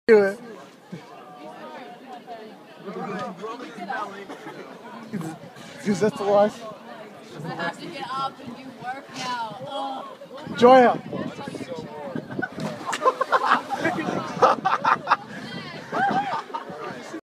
FULLY CLOTHED BELLY FLOP!
Kid does belly flop wearing everything at Latin Day MMXIII